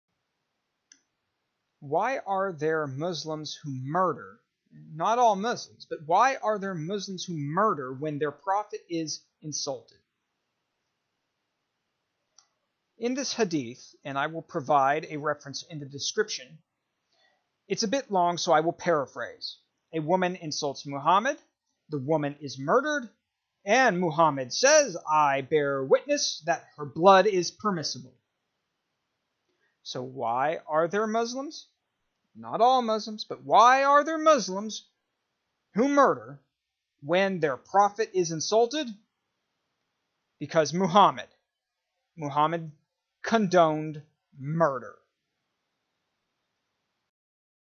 Insulting the prophet, Muhammad / Hadith, Islam's bane / Strength of Character / 20220516
Muhammad condoned murder.
Sunan an-Nasa’i 4070